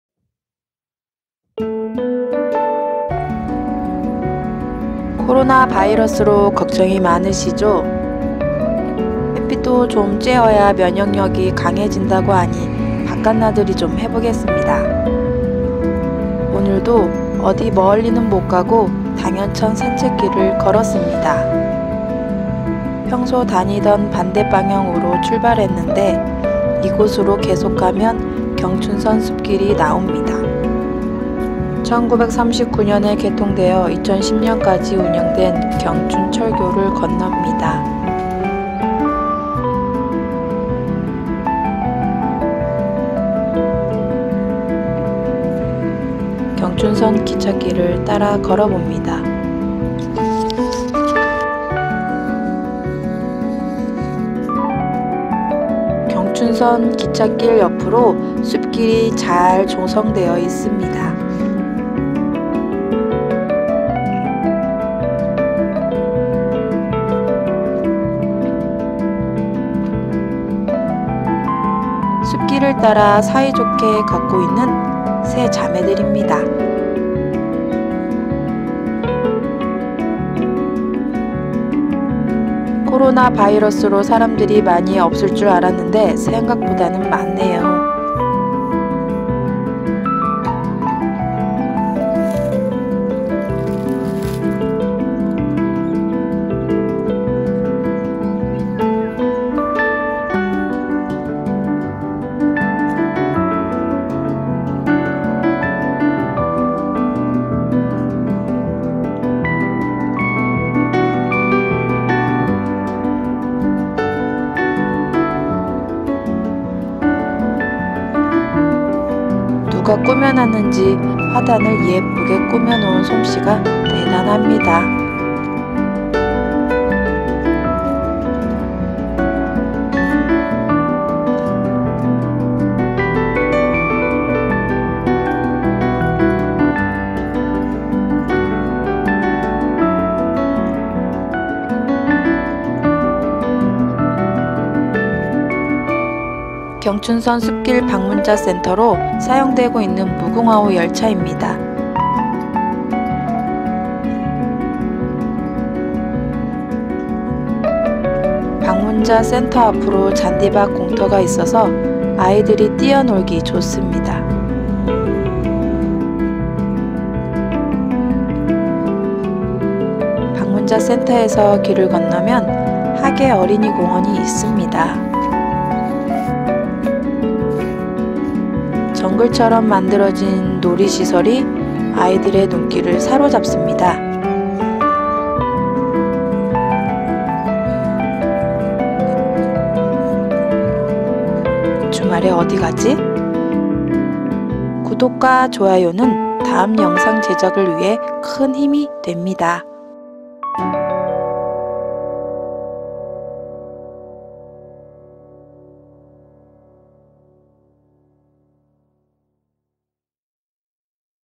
당현천 산책길을 따라 경춘선숲길 나들이 - 코로나 감염 대비 마스크 쓰고 산책하기
평소 당현천 산책길을 따라 걷다보면 경춘선 숲길로 갈 수 있습니다.
경춘철교를 지나 철길을 따라 걷다보면 철길 옆으로 만들어진 숲길이 보입니다.
코로나 바이러스러 모두들 마스크 쓰고 열심히 산책하고 운동도 했습니다.
코로나 바이러스의 위기를 모두 잘 이겨내고 건강하게 다시 만나요. ^^